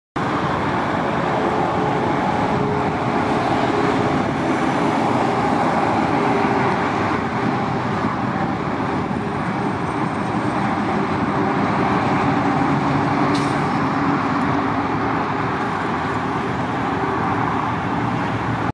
4KCC express way traffic
This is video is free to use. It is in creative commons. 4KCC video is a 4K movie. It was shot on a Lumix FZ1000 camera. Traffic on an express way. Look at trucks and cars. 30FPS normal speed and I have the original raw. It is a flat near gray image. If you need it, I can send it to you.